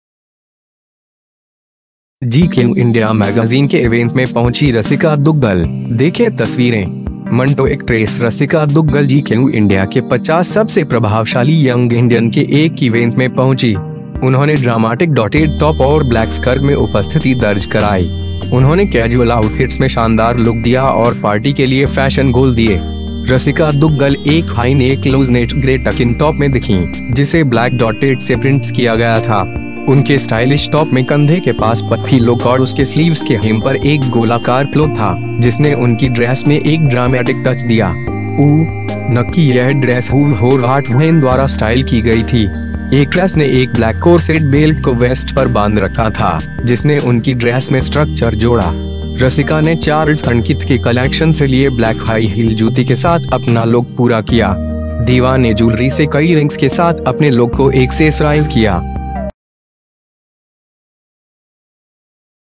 जीक्यू इंडिया मैगजीन के इवेंट में पहुंची रसिका दुगल, देखें तस्वीरें
for mouth watering Indian Food Recipes जीक्यू इंडिया मैगजीन के इवेंट में पहुंची रसिका दुगल, देखें तस्वीरें

मंटो' एक्ट्रेस रसिका दुगल जीक्यू इंडिया के 50 सबसे प्रभावशाली यंग इंडियन के एक इवेंट में पहुंची। उन्होंने ड्रामाटिक डॉटेड टॉप और ब्लैक स्कर्ट में उपस्थिति दर्ज कराई। उन्होंने कैजुअल आउटफिट्स में शानदार लुक दिया और पार्टी के लिए फैशन गोल दिए।

रसिका दुगल एक हाईनेक लूज़ नेट ग्रे टकइन टॉप में दिखीं, जिसे ब्लैक डॉटेड से प्रिंट्स किया गया था।

उनके स्टाइलिश टॉप में कंधे के पास पफी लुक और उसके स्लीव्स के हेम पर एक गोलाकार फ्लो था, जिसने उनकी ड्रैस में एक ड्रामैटिक टच दिया। उ

नकी यह ड्रैस 'हू व्होर व्हाट व्हेन' द्वारा स्टाइल की गई थी।

एक्ट्रैस ने एक ब्लैक कोर्सेट बेल्ट को वैस्ट पर बांध रखा था, जिसने उनकी ड्रैस में स्ट्रक्चर जोड़ा। रसिका ने चार्ल्स एंड कीथ के कलैक्शन से लिए ब्लैक हाई हील जूती के साथ अपना लुक पूरा किया। दीवा ने जूलरी से कई रिंग्स के साथ अपने लुक को एक्सेसराइज़ किया।